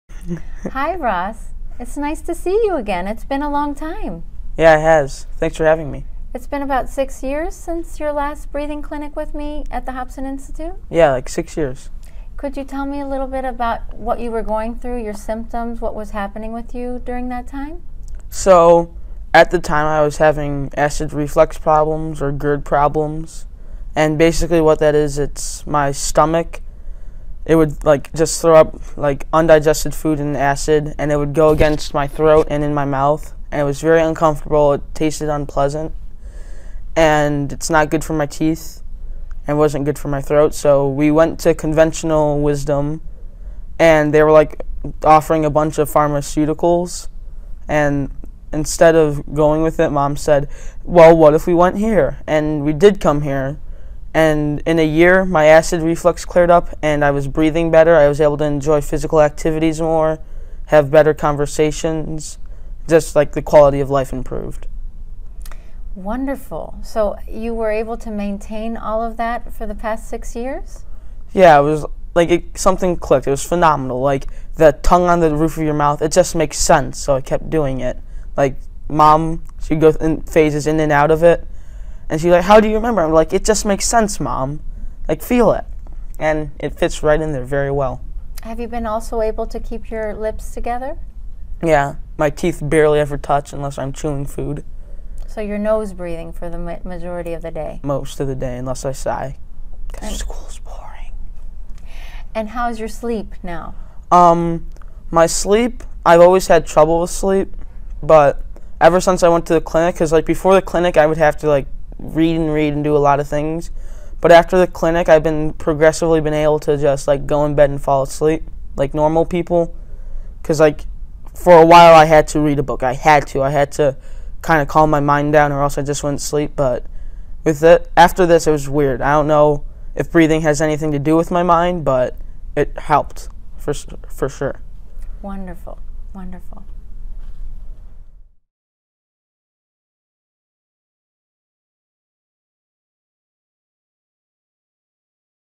Real People Tell Their Breathing Breathing Story: 14 year old Ross
Watch former patient Ross, explain the aspects of the breathing training that greatly improved his acid reflux and quality of life.  If you are interested in what Ross went through you can sign up